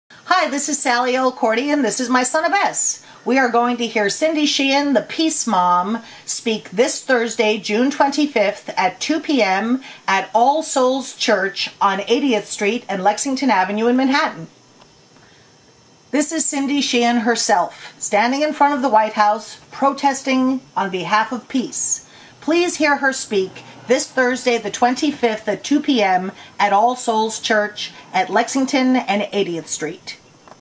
"Peace Party for Cindy Sheehan" ~ June 2009
Sallie O. Elkordy produced and hosted speeches by 30 activists and entertainers from All Souls Church to the United Nations Church culminating each with speeches by Cindy Sheehan, "The Peace Mom" about her online booklet "Myth America: 10 Myths of the Robber Class and the 'Casey' for Revolution!".  to get your copy.  Special thanks to Elaine Brower for her immeasurable contributions toward marketing and more.